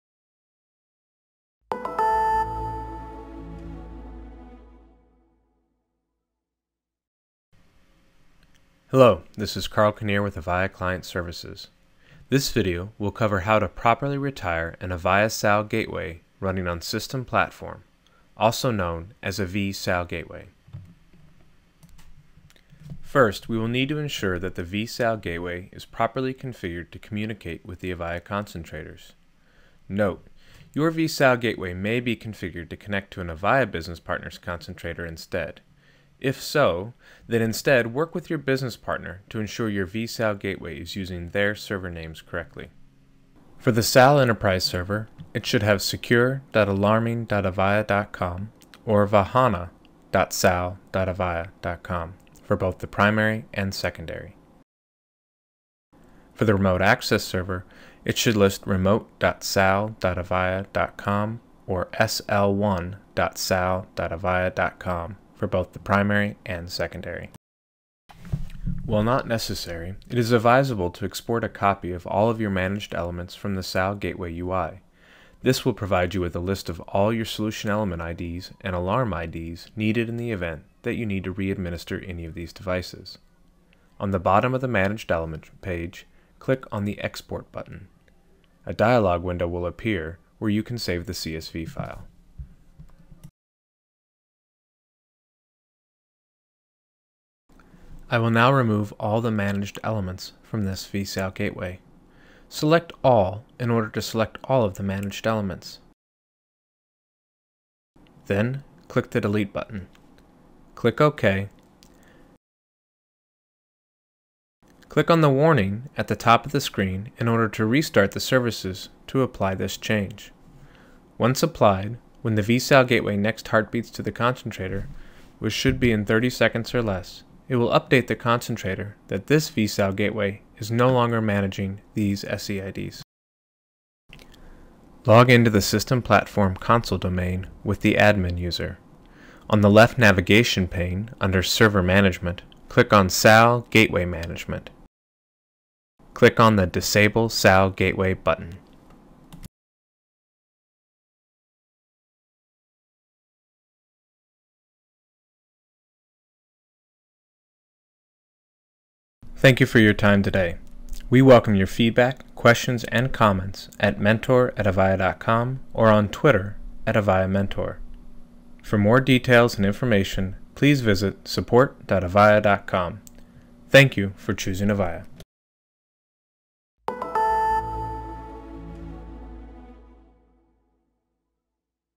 How to Retire an Avaya SAL Gateway on System Platform (VSALGW)
This video shows how to properly retire an Avaya SAL Gateway running on System Platform, also known as a VSALGW. This includes how to properly remove managed elements and then disable the Services VM.
Produced by: Carl Knerr